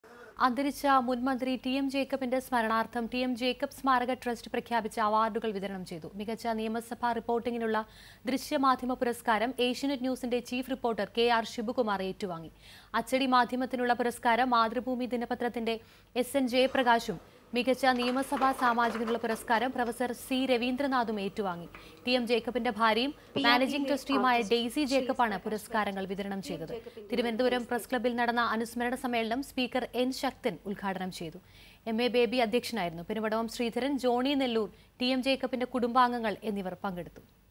TM Jacob Memorial Trust Award for Best Assembly Reporting gets Asianet News reporter Shibu Kumar
TM Jacob Memorial Trust Award presentation cermony best Assembly reporting award gets Asianet News report Shibu Kumar

ടി എം ജേക്കബ് സ്മാരക അവാര്‍ഡ് ഷിബു കുമാറിന് സമ്മാനിച്ചു